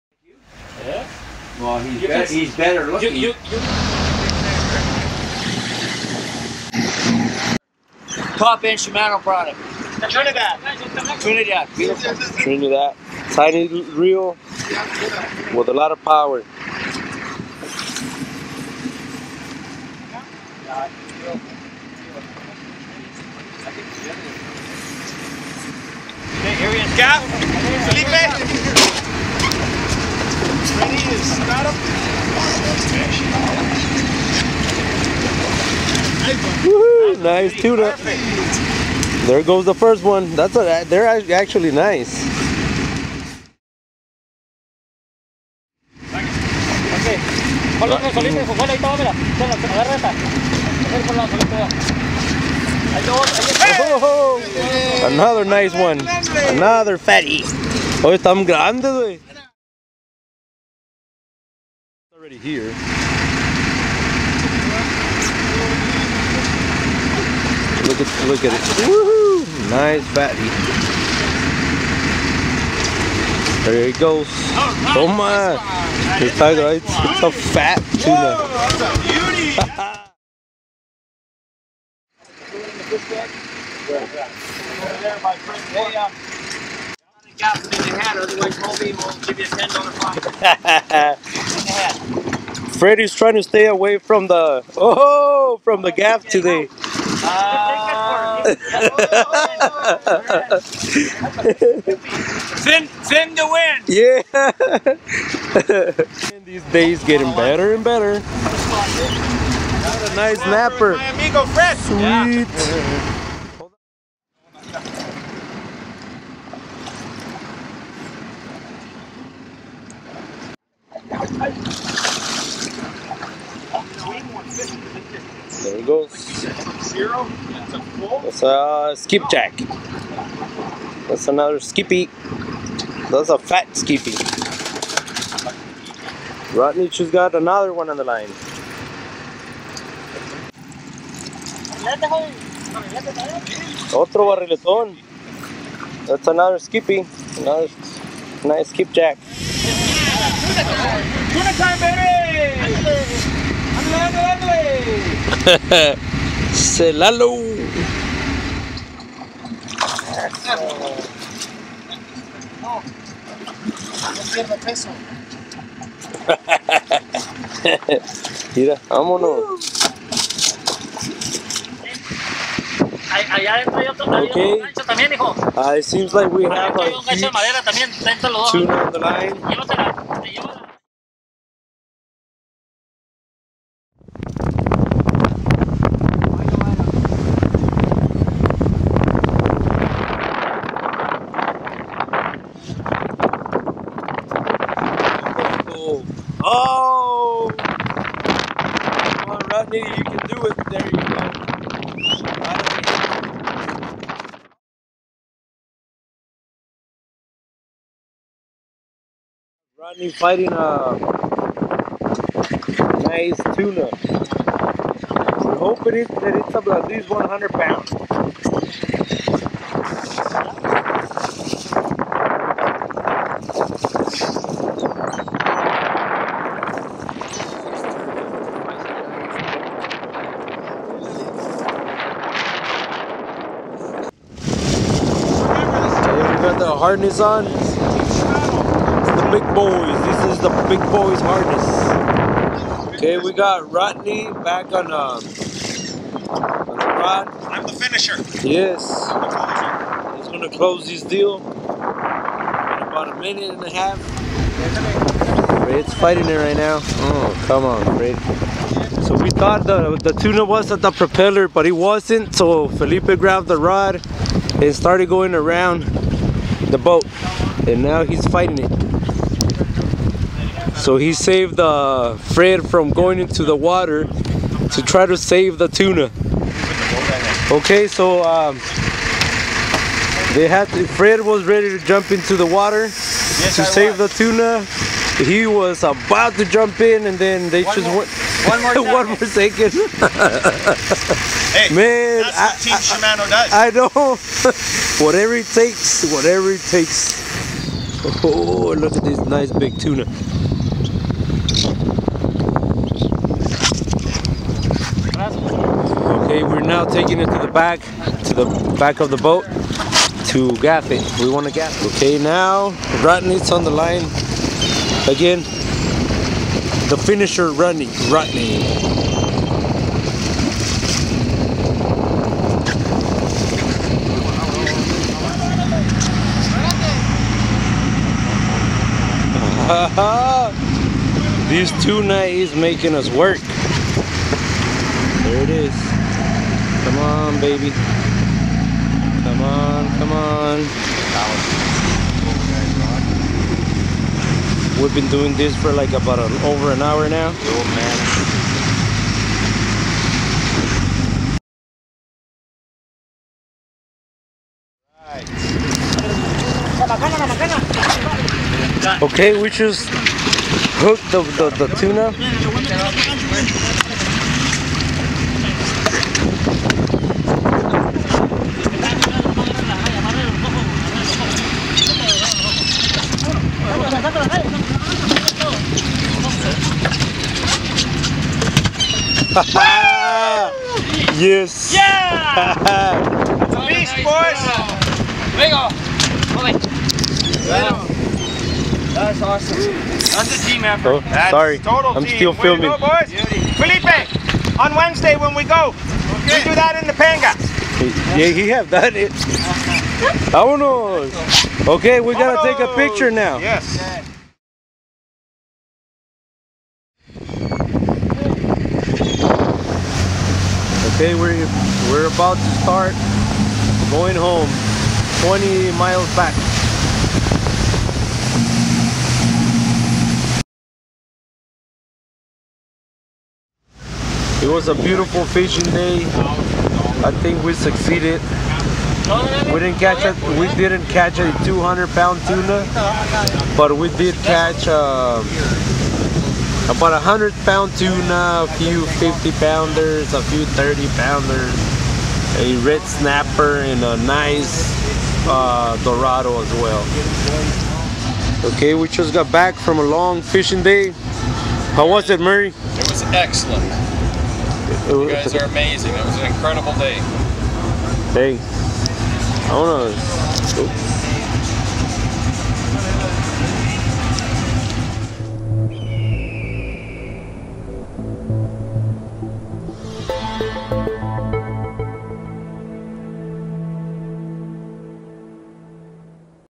Fishing in Cabo San Lucas
Fishing in Cabo San Lucas with team Tag Cabo Sportfishing.